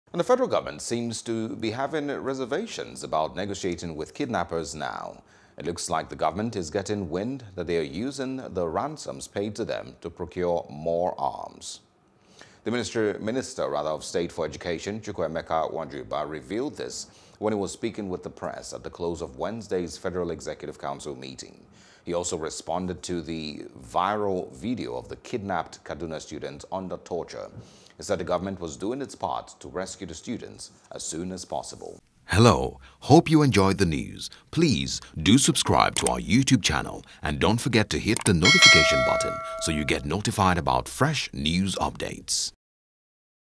Fighting Banditry: No More Talks With Culprits – FG
The Federal Government seems to be having reservations about negotiating with kidnappers now.